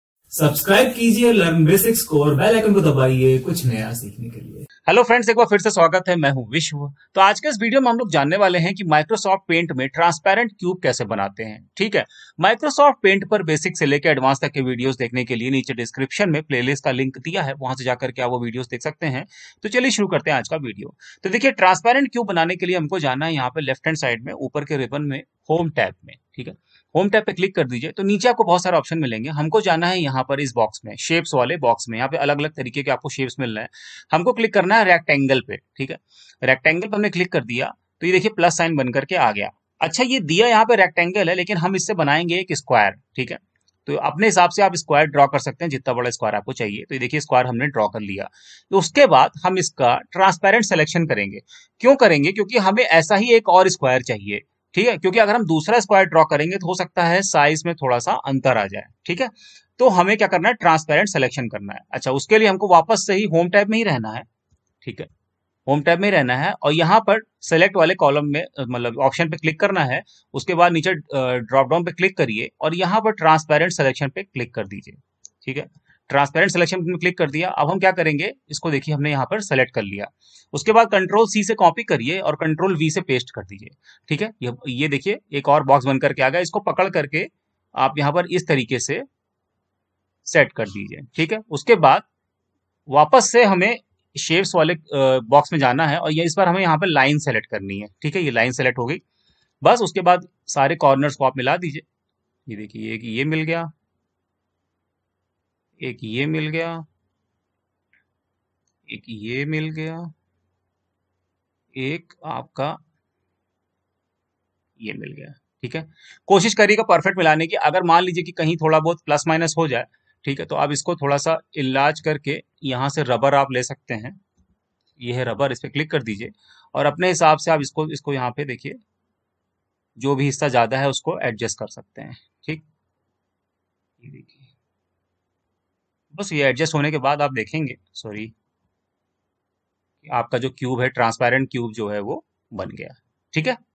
How to make transparent cube in Microsoft paint ?
In this video we will learn that how to make transparent cube in Microsoft paint?
How do you make a cube in ms paint ?

I will upload some more basics of MS paint transparent cube in my upcoming videos. If you need any help with the related topic so kindly write it on comment section.

Keep watching.
Keep learning.